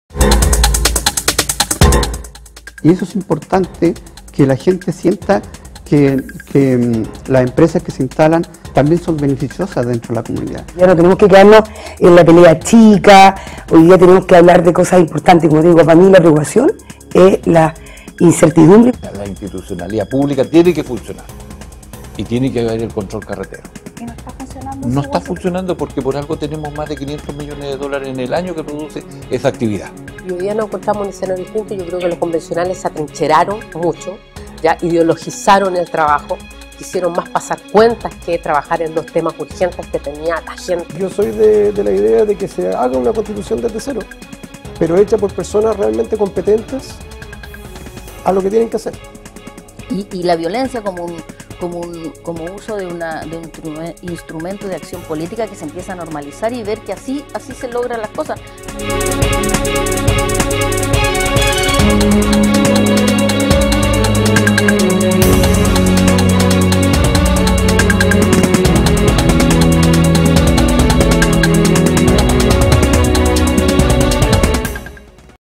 Diálogos parlamentarios: "Hablemos de presente y futuro"
Un programa de conversación permanente con los Senadores y Diputados de nuestra región, para hablar sobre temáticas de preocupación para el desarrollo sostenible del Biobío.